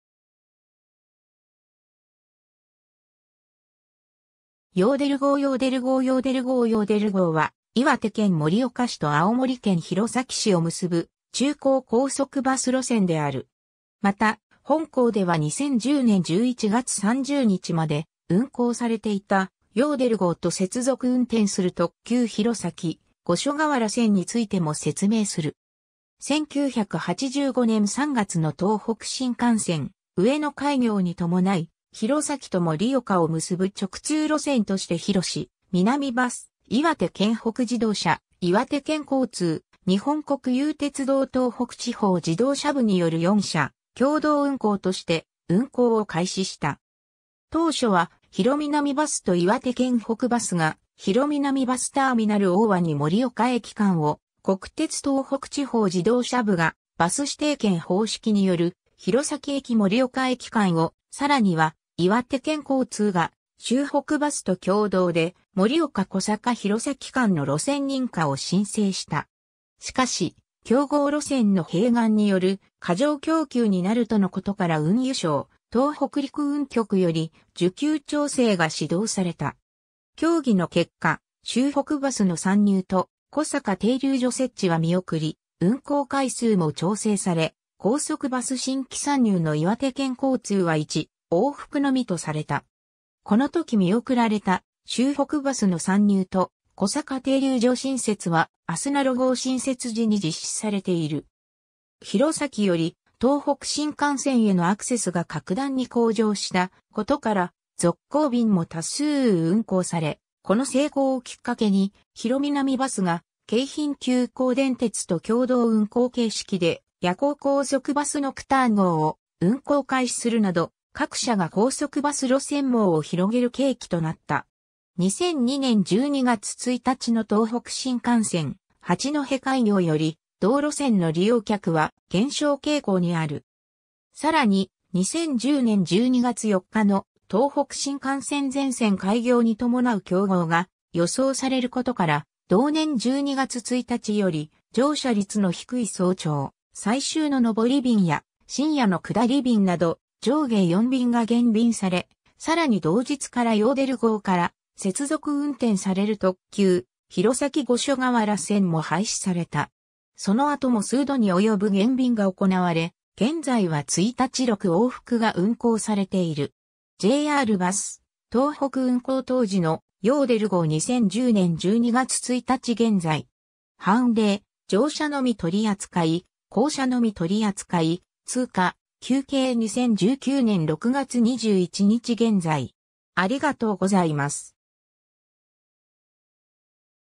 ヨーデル号
ヨーデル号（弘南バス） ヨーデル号（岩手県北バス） ヨーデル号（岩手県交通） ヨーデル号（ヨーデルごう）は、岩手県盛岡市と青森県弘前市を結ぶ昼行高速バス路線である。
また、本項では2010年11月30日まで運行されていた、ヨーデル号と接続運転する特急弘前 - 五所川原線についても説明する。
 1985年3月の東北新幹線上野開業に伴い、弘前と盛岡を結ぶ直通路線として弘南バス、岩手県北自動車（岩手県北バス）、岩手県交通、日本国有鉄道東北地方自動車部（国鉄バス、現・JRバス東北）による4社共同運行として運行を開始した。
当初は弘南バスと岩手県北バスが弘南バスターミナル - 大鰐 - 盛岡駅間を、国鉄東北地方自動車部が「バス指定券」方式による弘前駅 - 盛岡駅間を、さらには岩手県交通が秋北バスと共同で盛岡 - 小坂 - 弘前間の路線認可を申請した。
しかし、競合路線の併願による過剰供給になるとのことから運輸省東北陸運局（当時、現・国土交通省東北運輸局）より需給調整が指導された。
協議の結果、秋北バスの参入と小坂停留所設置は見送り、運行回数も調整され、高速バス新規参入の岩手県交通は1往復のみとされた。
この時見送られた秋北バスの参入と小坂停留所新設はあすなろ号新設時に実施されている。
 弘前より東北新幹線へのアクセスが格段に向上したことから、続行便も多数運行され、この成功をきっかけに弘南バスが京浜急行電鉄（当時）と共同運行形式で夜行高速バスノクターン号（弘前 - 品川バスターミナル）を運行開始するなど、各社が高速バス路線網を広げる契機となった。
 2002年12月1日の東北新幹線八戸開業より同路線の利用客は減少傾向にある。
 さらに、2010年12月4日の東北新幹線全線開業に伴う競合が予想されることから、同年12月1日より乗車率の低い早朝・最終の上り便や深夜の下り便など上下4便（2往復）が減便され、さらに同日からヨーデル号から接続運転される特急｢弘前 - 五所川原線｣も廃止された。
その後も数度に及ぶ減便が行われ、現在は1日6往復が運行されている。
 JRバス東北運行当時の「ヨーデル号」 2010年12月1日現在。
 凡例： ○：乗車のみ取り扱い、●：降車のみ取り扱い、↓↑：通過、◇◆：休憩 2019年6月21日現在。